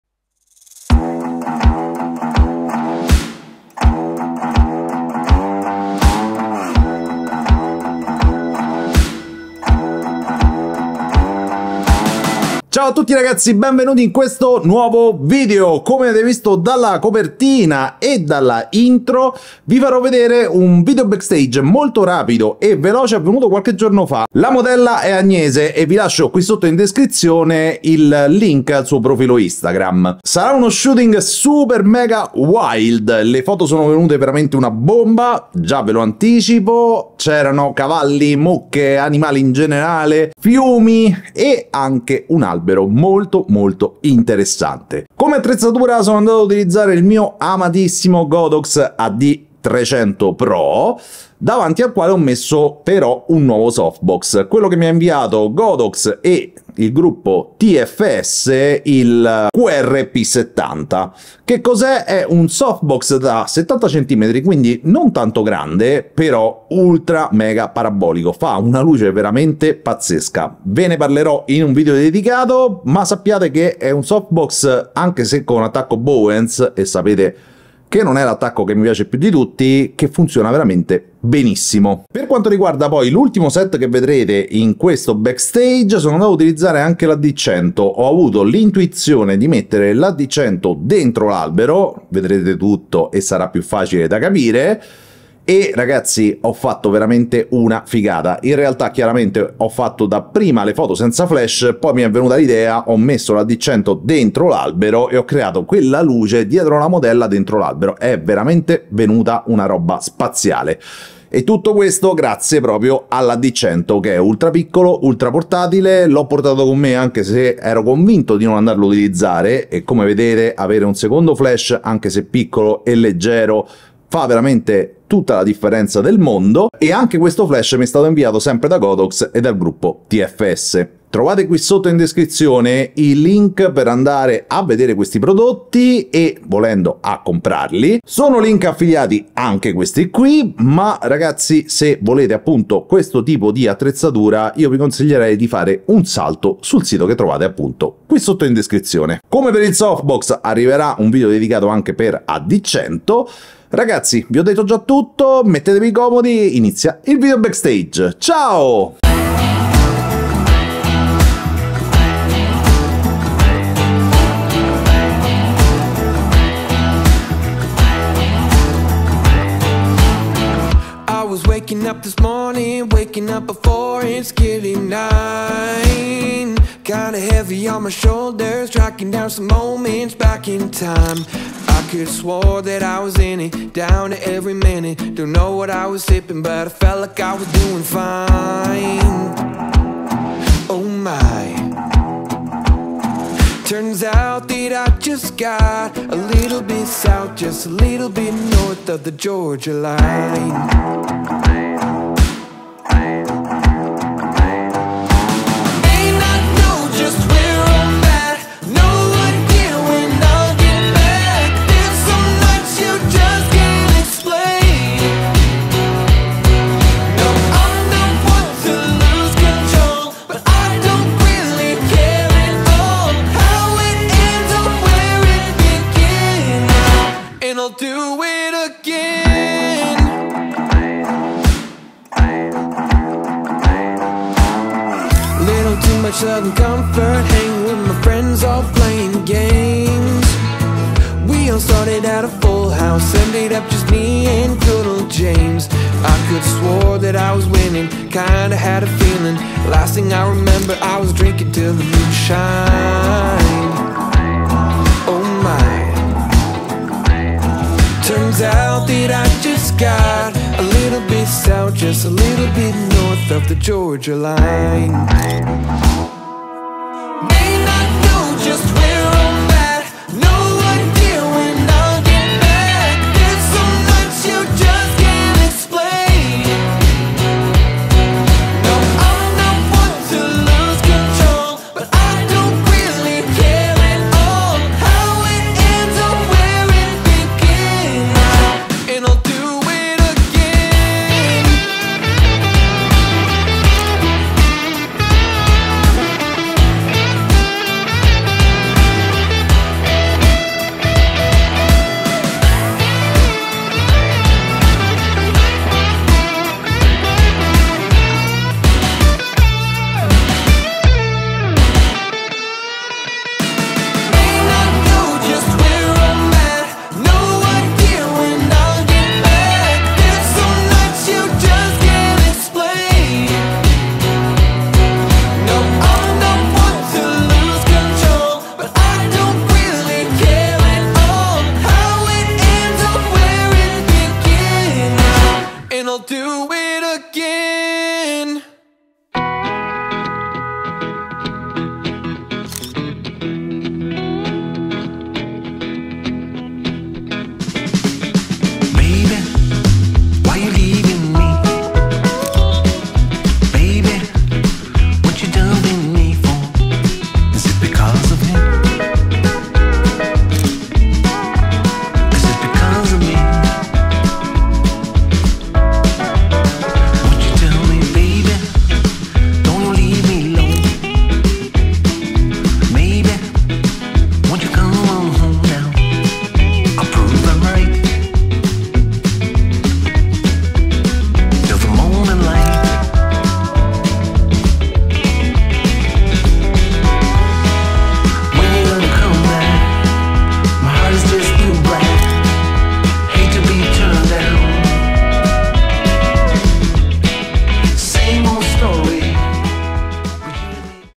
Summer Wild bts with godox AD300 PRO
Ciao ragazzi ecco un nuovo video backstage, fatemi sapere con un commento cosa ne pensate!

AS5

Attrezzatura utilizzata per questo video:

Iphone 14 Pro:

Sony A7 IV:
Sigma 85 1.4 for Sony:
Sigma 35 1.4 for Sony:
Godox AD300pro:
Godox AD100pro:
Gvm SD80D: